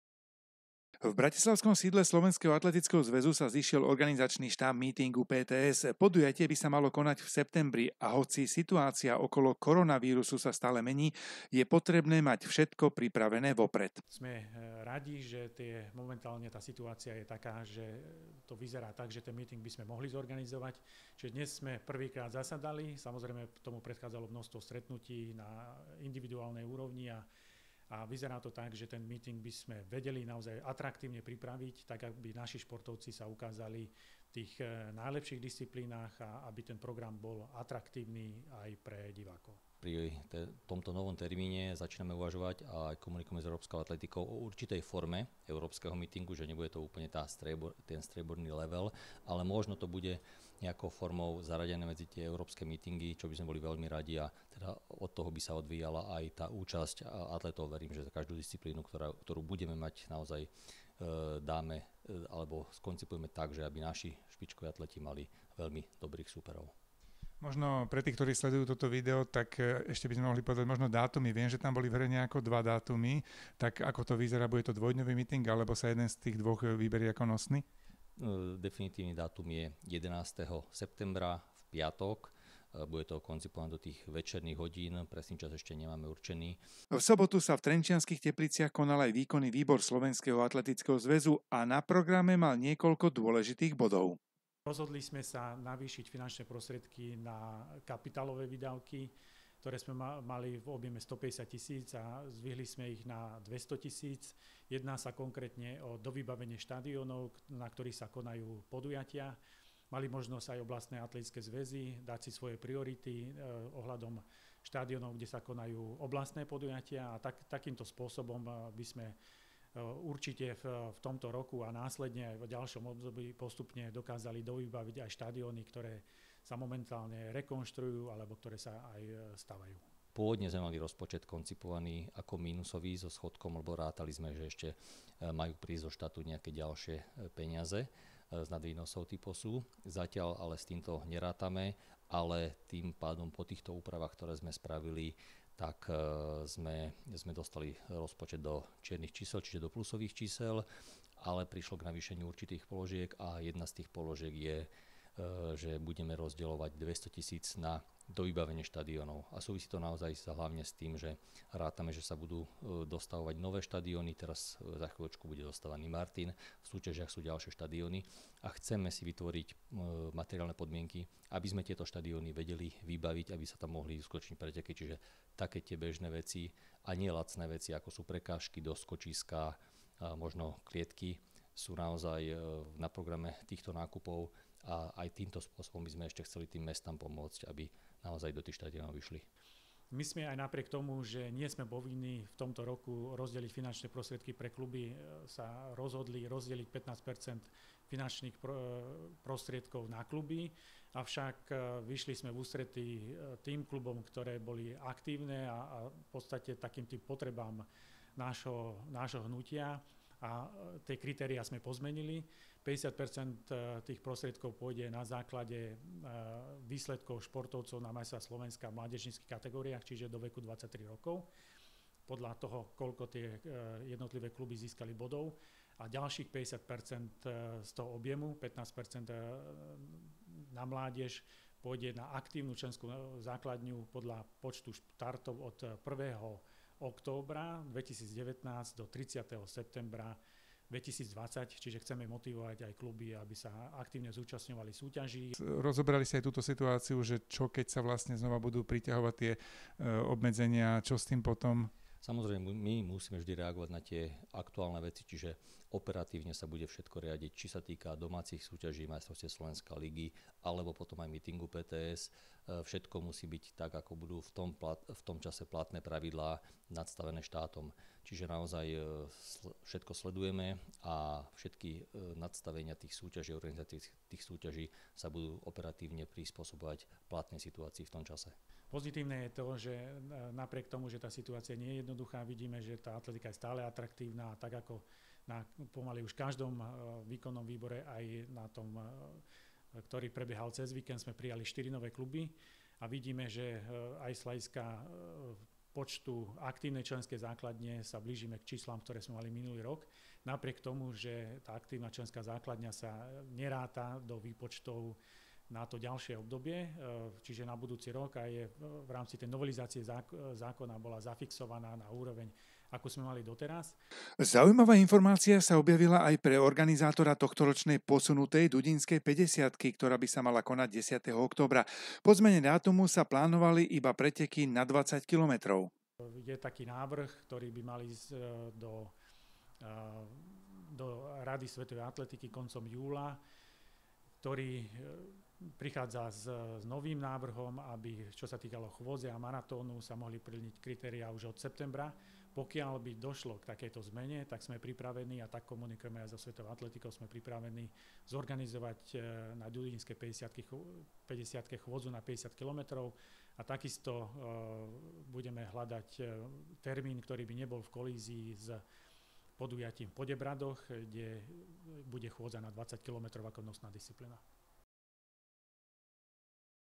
PTS prípravné stretnutie, 13 7 2020
O dva mesiace by sa mal v Šamoríne konať míting P-T-S. V sídle SAZ v Bratislave zasadal organizačný štáb. Vo videu ale nájdete i niekoľko informácii z výkonného výboru, ktorý sa konal v Trenčianskych Tepliciach.